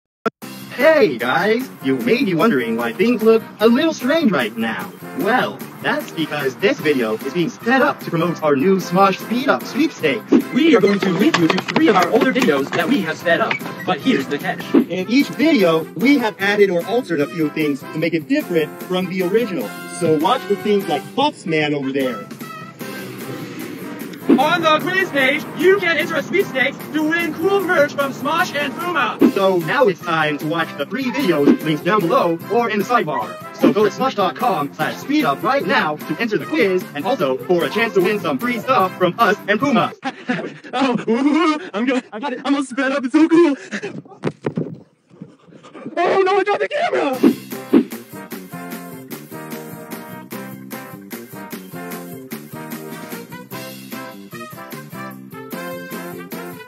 Smosh - Speedup Sweepskates
©️2009 Smosh

Watch and learn how to take the quiz and enter the Speedup Sweeps!